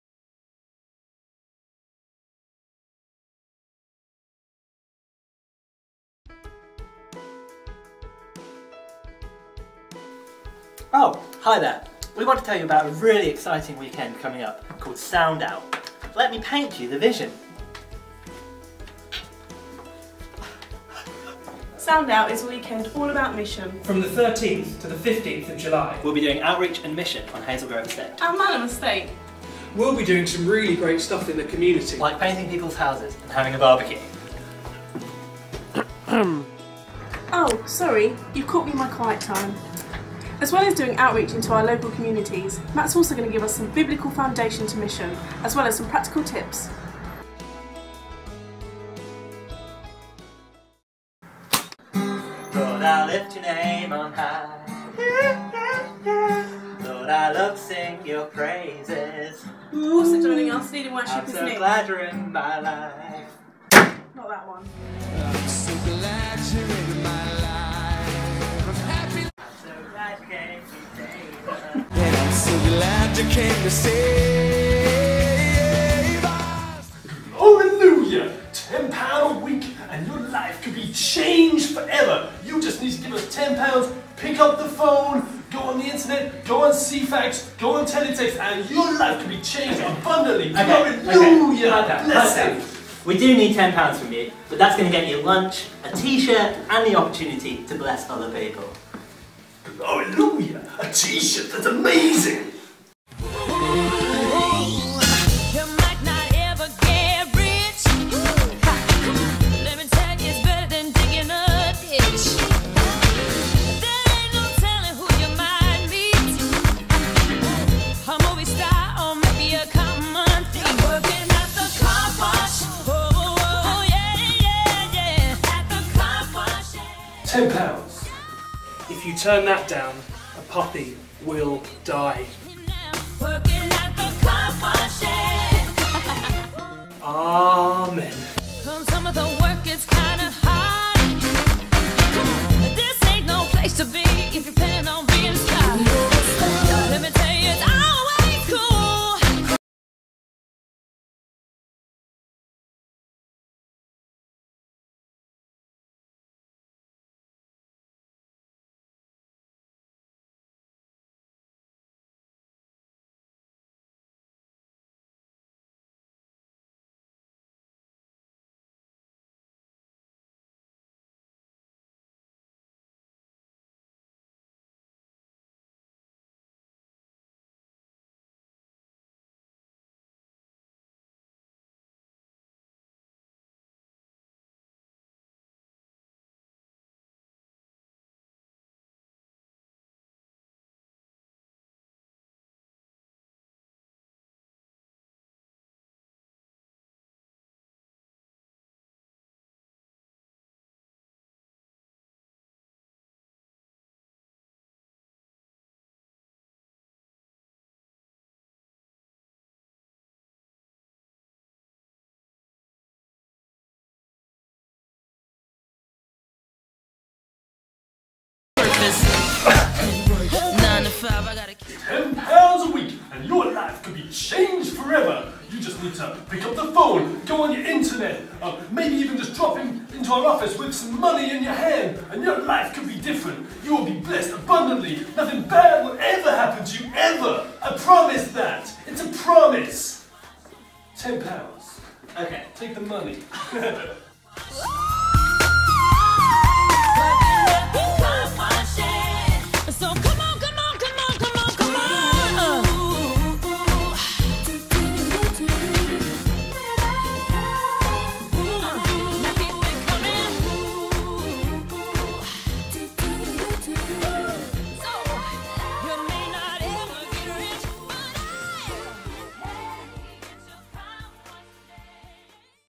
SoundOut 2012
A weekend of mission, teaching, worship and prayer with Sound.